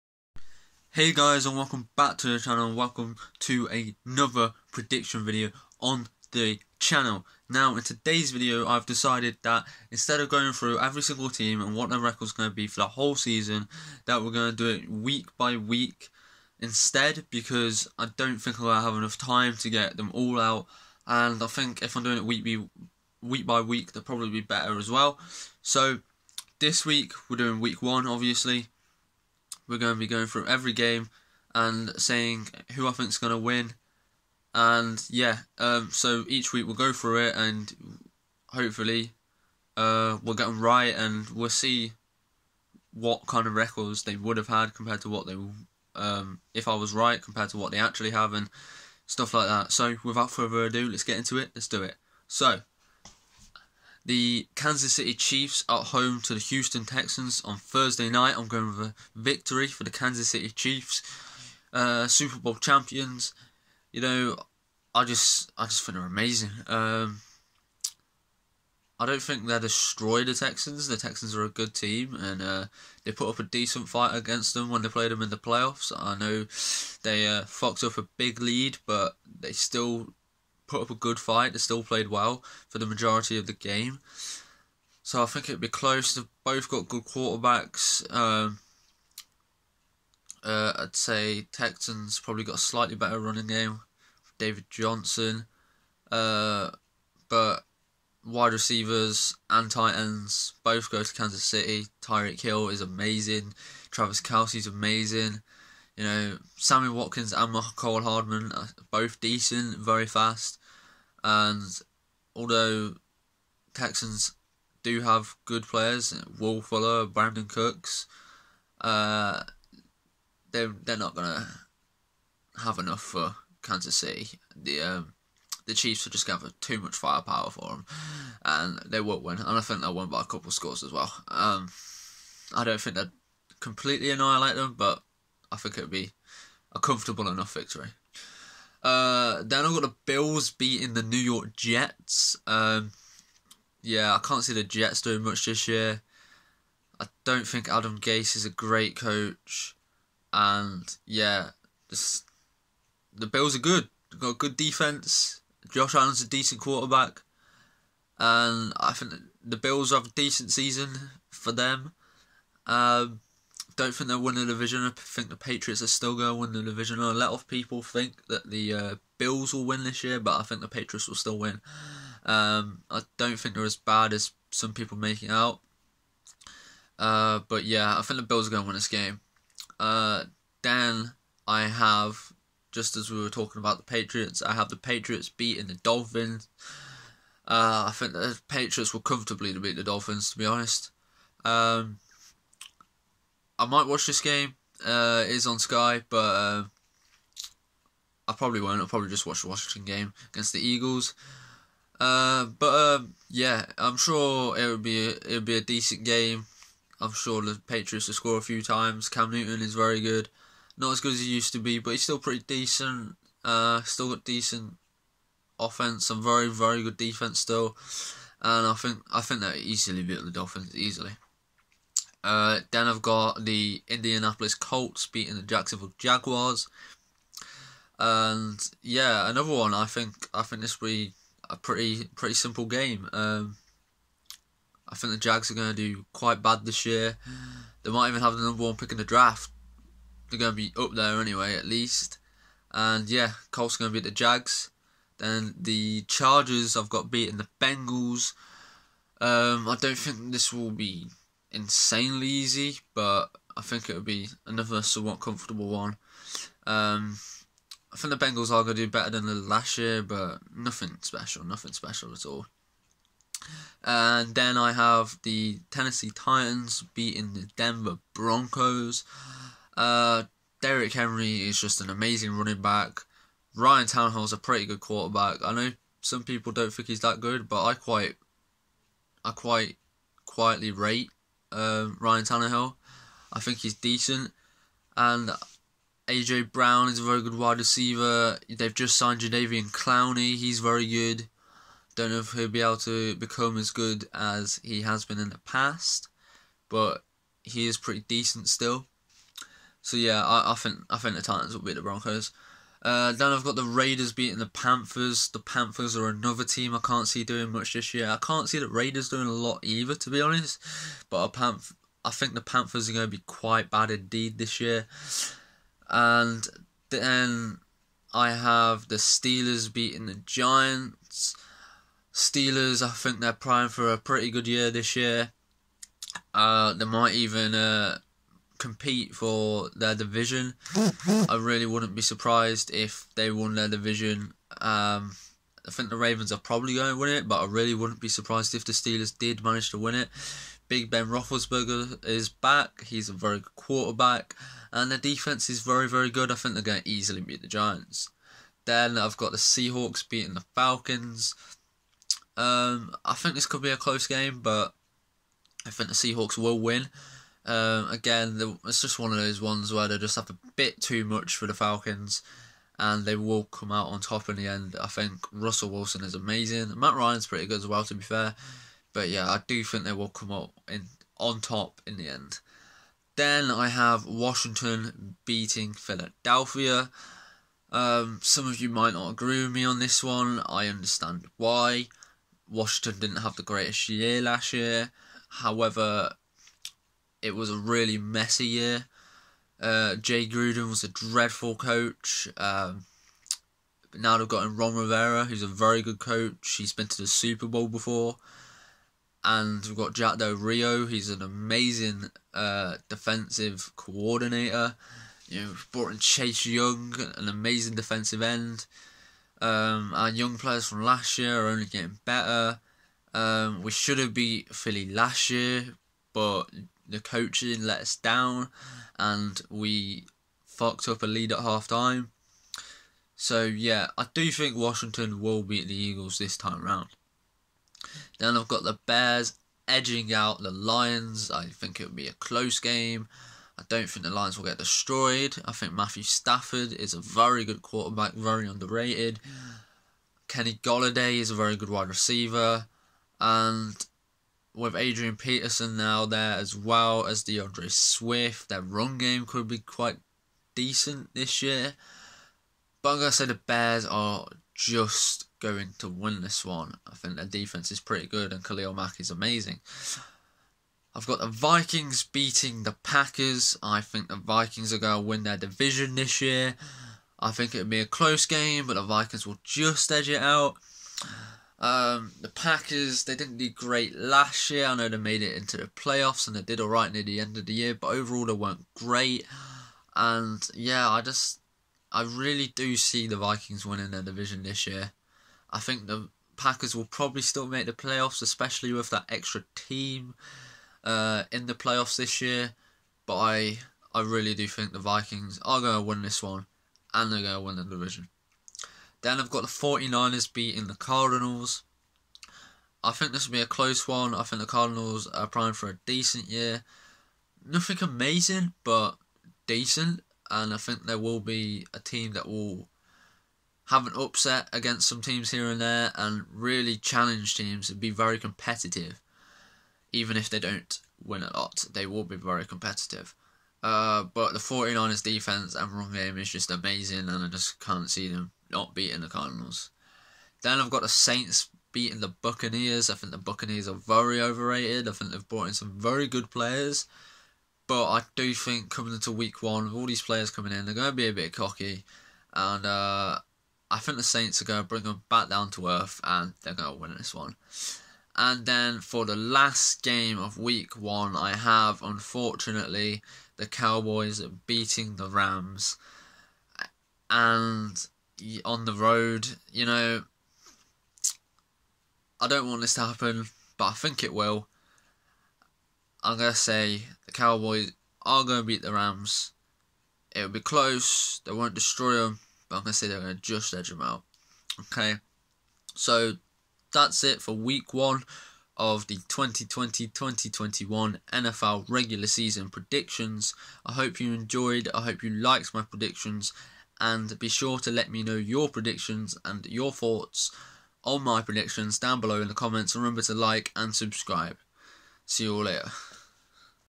Nfl Week 1 regular season predictions!
Hi guys in this video I have my nfl regular season predictions for week 1 I hope you enjoy and like my predictions be sure to tell me your thoughts in the comments below!